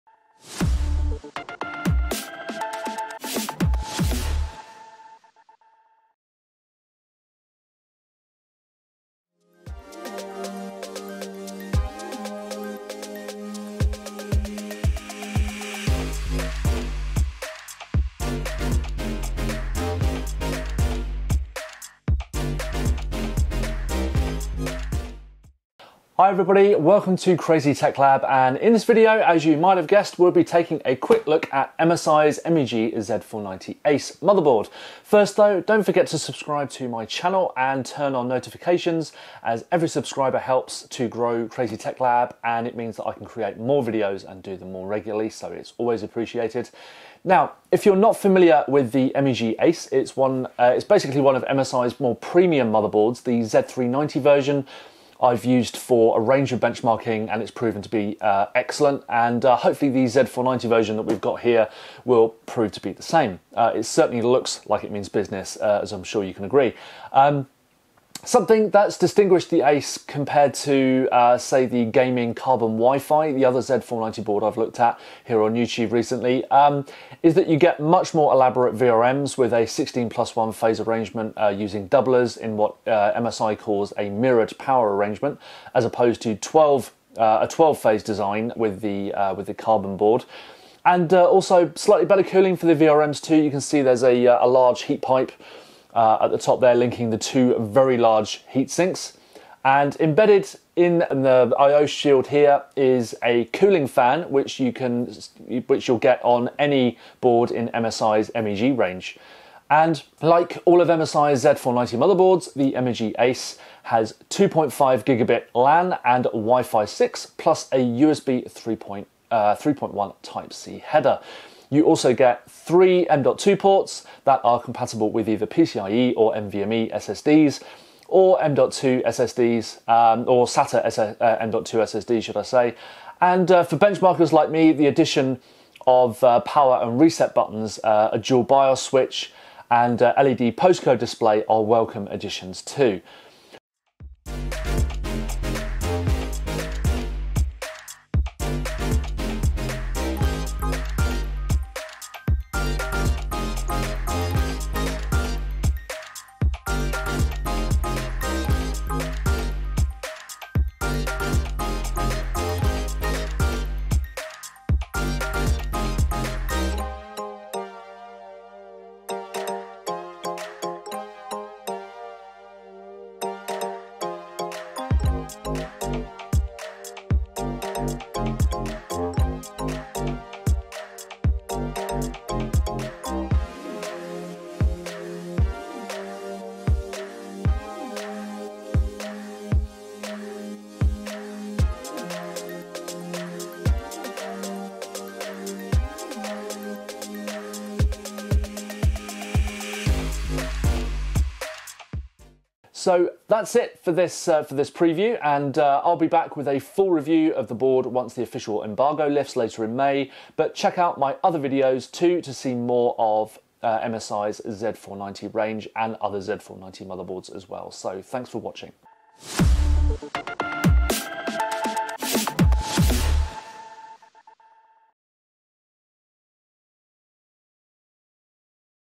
MSI MEG Z490 Ace Hand On Preview!
I take a close look at MSI's MEG Z490 Ace - a premium ATX board from MSI for Intel's 10th Gen CPUs